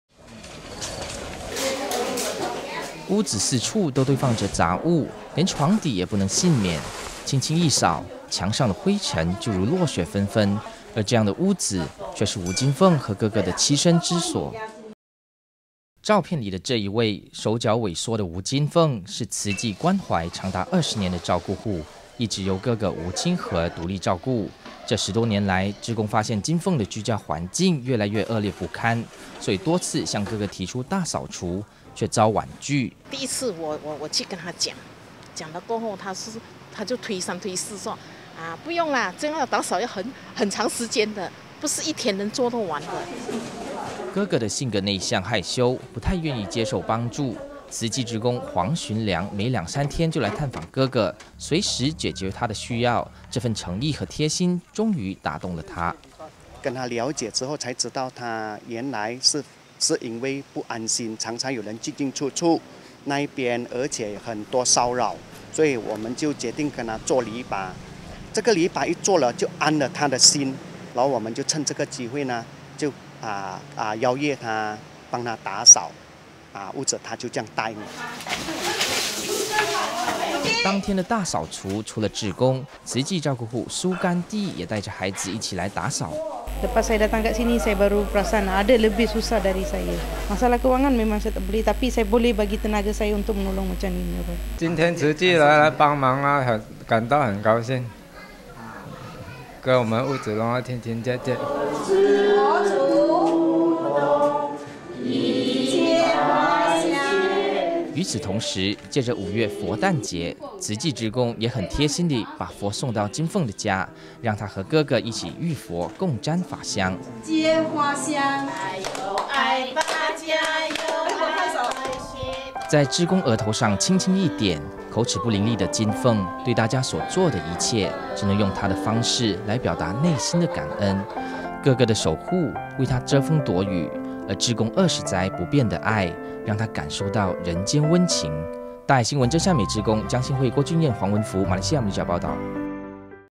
二十載不變的承諾 志工為金鳳打掃浴佛
屋子四處都堆放著雜物，連床底也不能倖免，輕輕一掃，牆上的灰塵就如落雪紛紛，而這樣的屋子，卻是吳金鳳和哥哥的棲身之所。

照片裡的這一位，手腳萎縮的吳金鳳，是慈濟關懷長達二十年的照顧戶，一直由哥哥吳清和獨立照顧。這十多年來，志工發現金鳳的居家環境越來越惡劣不堪，所以多次向哥哥提出大掃除，卻遭婉拒。

哥哥的性格內向又害羞，不太願意接受幫助，慈濟志工黃循糧每兩，三天就來探訪哥哥，隨時解決他的需要，這份誠意和貼心終於打動了他。

當天的大掃除，除了志工，慈濟照顧戶蘇甘蒂也帶著孩子一起來打掃。

與此同時，藉著五月佛誕節，慈濟志工也很貼心地，把琉璃佛送到金鳳家，讓她和哥哥一起浴佛共沾法香。

在志工額頭上輕輕一點。口齒不伶俐的金鳳，對大家所做的一切，只能用她的方式來表達內心的感恩。哥哥的守護，為她遮風躲雨，而志工二十載不變的愛，讓她感受到人間溫情。